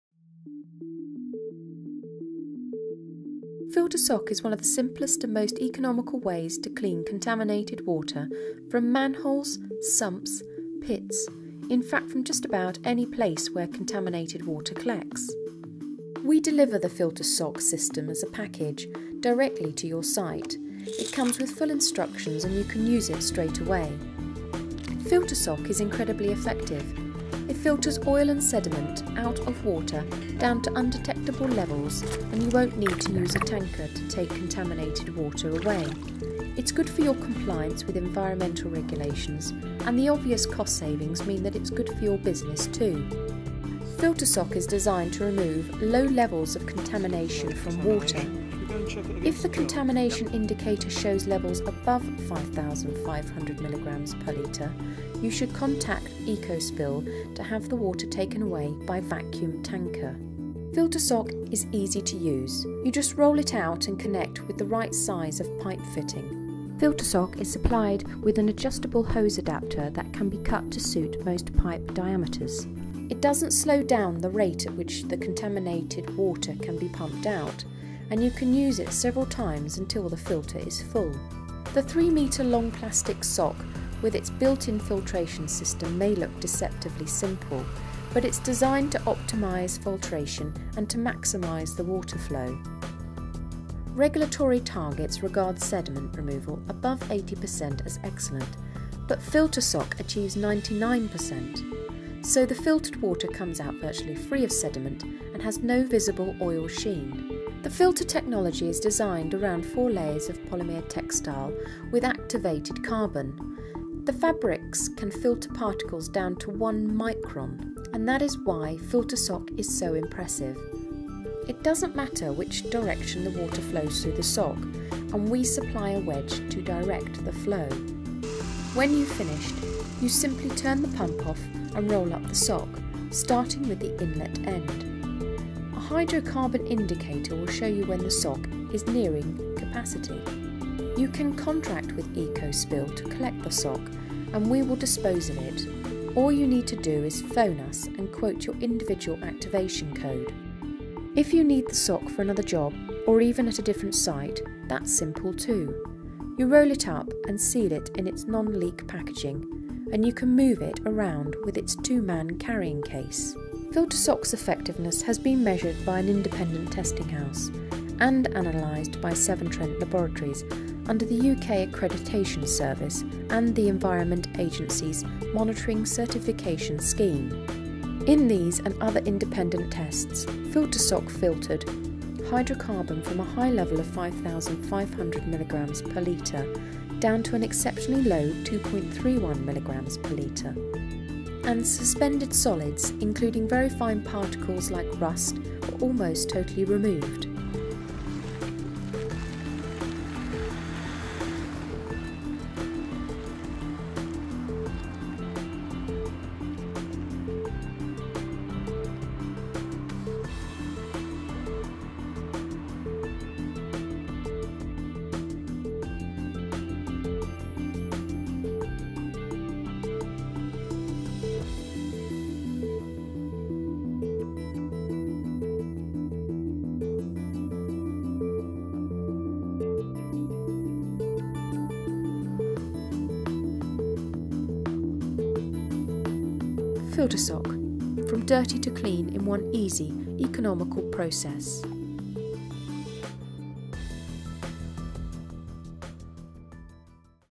Ecospill Filtasock - An Innovative Filtration System
Filtasock is an innovative filtration system which offers significant economic and environmental benefits. The Filtasock filtration system is one of the most simple and economical ways of dealing with contaminated water being pumped out of manholes, vaults, sumps, bunds, retention ponds and ditches.

It uses multistage filtration to remove hydrocarbons and sediment to almost non-detectable levels. Extremely efficient, it can be used in a wide range of de-watering applications without slowing up the pumping process. Good for your business and good for the environment.

The Filtasock filtration system means that you don't have to take contaminated water away by tanker for cleaning elsewhere. It also means that you can avoid releasing contaminated water into the environment. Offering both cost savings and a way of reducing tanker traffic and pollution risks, Filtasock is especially attractive to companies registered for BSN EN ISO 14001 certification.